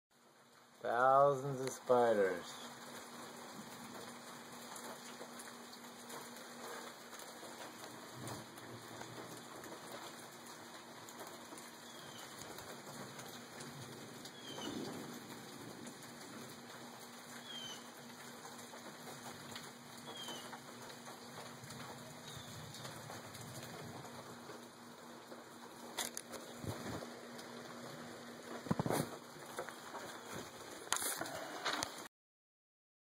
John found baby spiders eating big spider
Thousands of baby spiders... simple clean.org safe on dogs cats birds humans but kills fleas ticks spiders insects